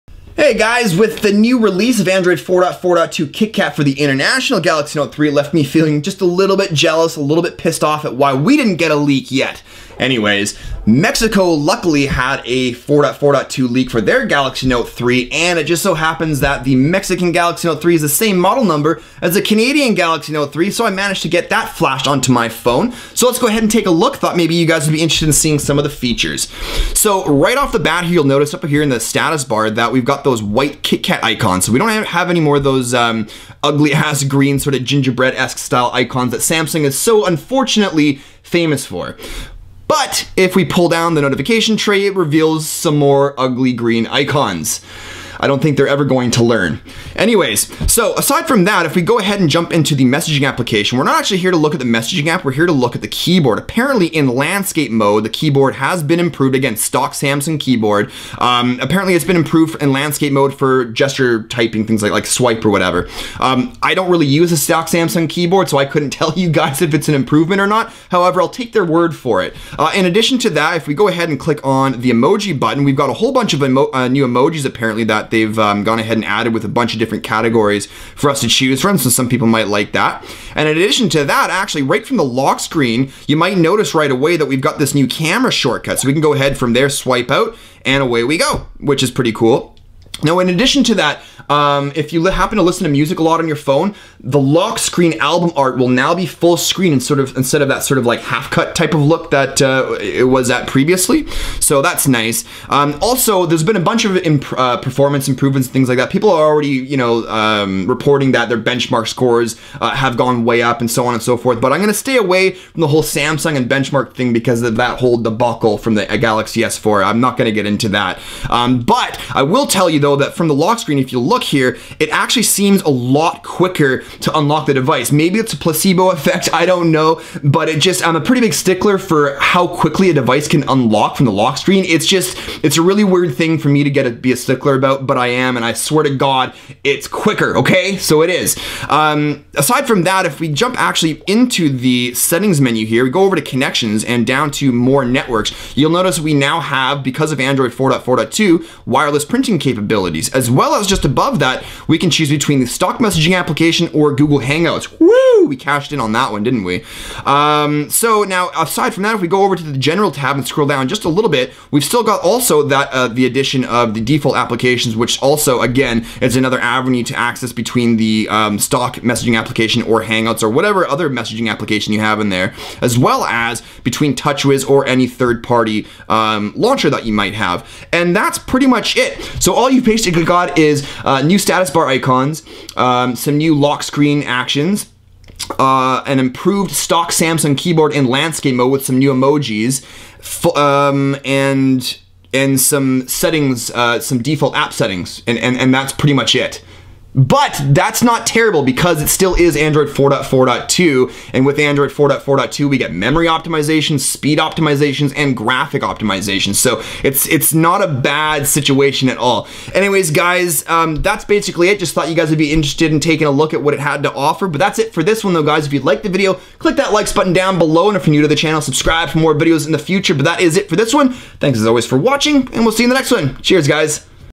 Samsung Galaxy Note 3 Android 4.4.2 KitKat New Features
Taking a look at the new features and changes that you can expect with Android 4.4.2 on your Galaxy Note 3.
For those of you that feel experienced enough to flash this on your own, here's the link to the files needed